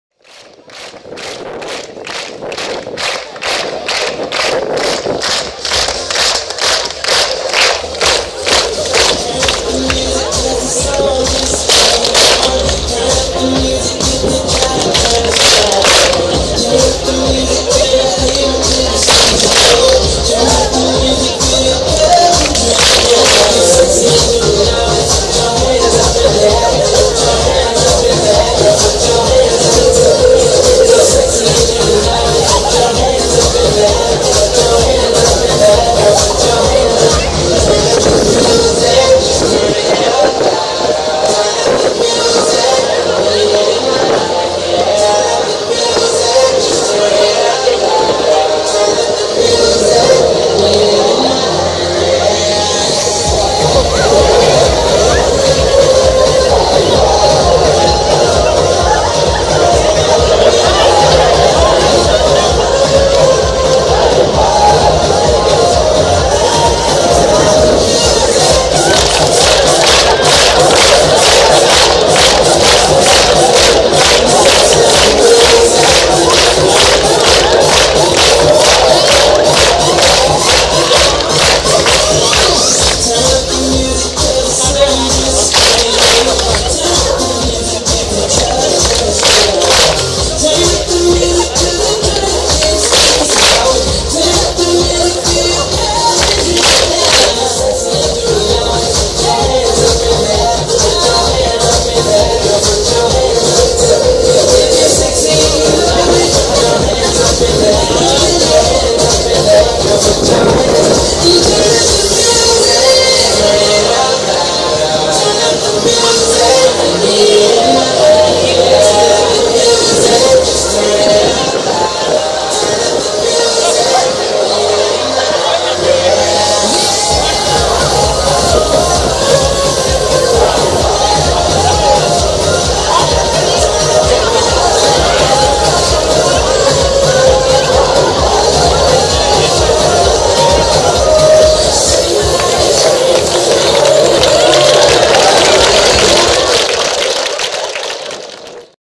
BLI KJENT DAG 2015 SPJELKAVIK VGS
Dans for elever og ansatte, på starten av "Bli kjent dagen"